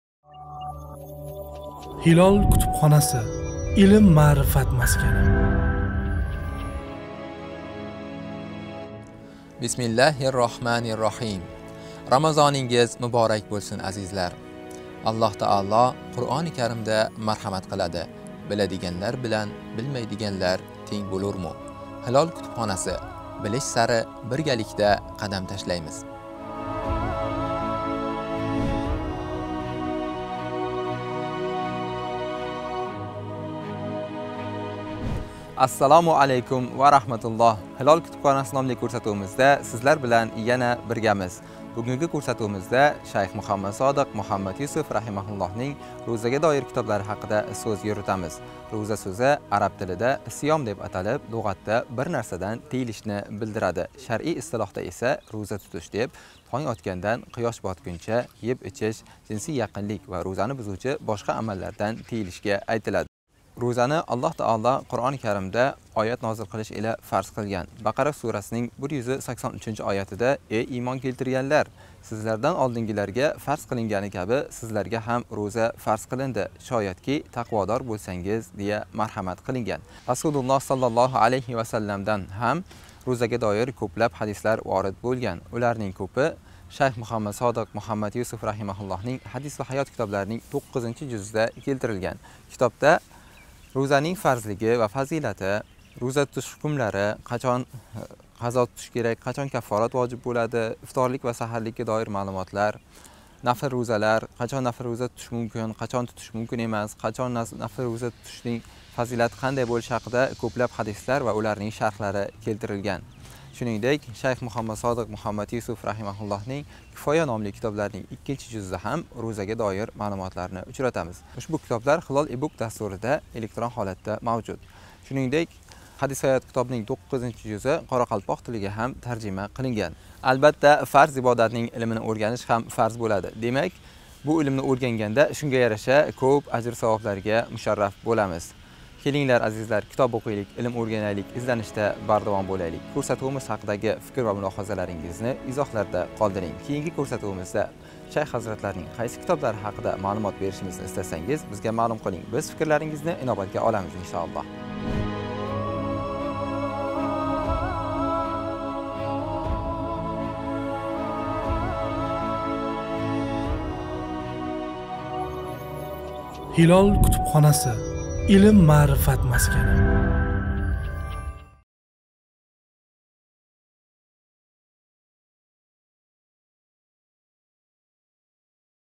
«Ro'za» kitobi - «Hilol» kutubxonasi
Qadrli kitobxon! Oʻzingizga maʼlumki, koʻpchiligimiz har yili Ramazoni sharif boshimizga soyabon boʻlib kelganida ul muborak oyning fazlidan bahramand boʻlib qolish uchun roʻza ibodati va uning ahkomlarini tuzukroq anglab yetishga harakat qilib qolamiz. «Hadis va hayot» silsilasining toʻqqizinchi juzi boʻlmish ushbu «Roʻza kitobi» siz bilan biz istagan va izlab yurgan koʻpgina maʼlumotlarni oʻz ichiga olgandir.

«Hilol» kutubxonasining ushbu sonida fazilatli shayx Muhammad Sodiq Muhammad Yusuf hazratlarining «Hadis va hayot» silsilasiga kiruvchi «Ro'za» kitobi haqida so'z boradi.

☎️ Ma'lumot uchun: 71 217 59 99